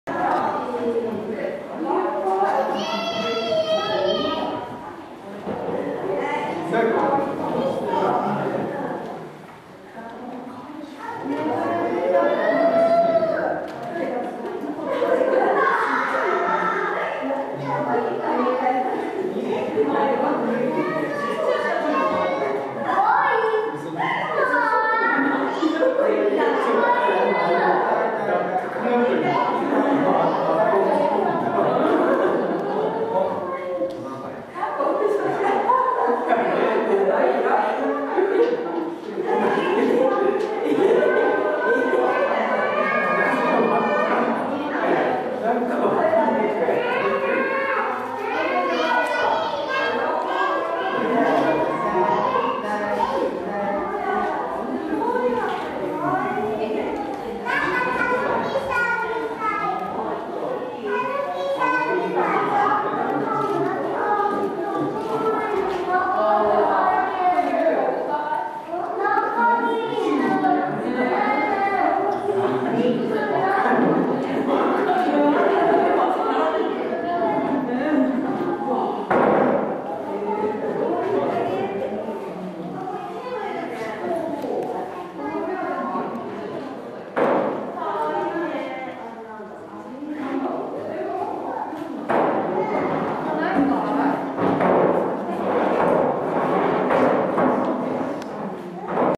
キン、天井につきました☆円山動物園レッサーパンダ
室内で木登りするキン、仔パンダでも天井につきました。

２０１３年１月１２日　円山動物園レッサーパンダ